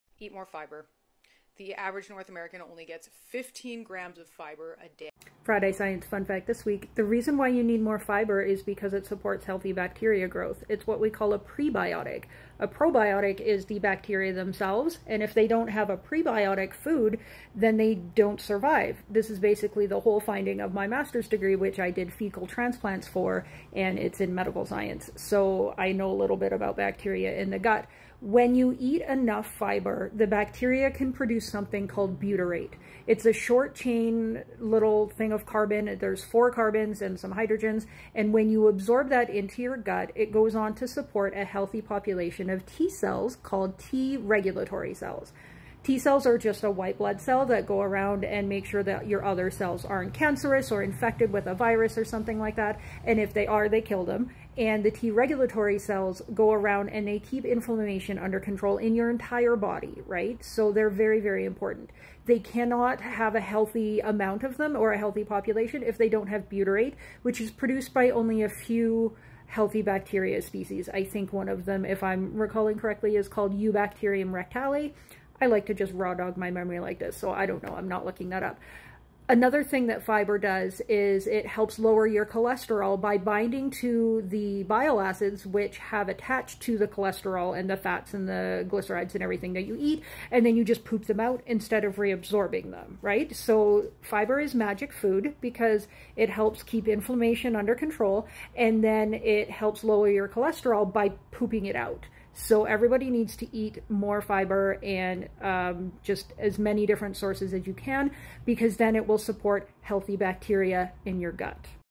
Friday Science Fun Fact Oct 3
Fiber is magic food. I shoveled pig shit in a barn for 2 years to bring you this fact.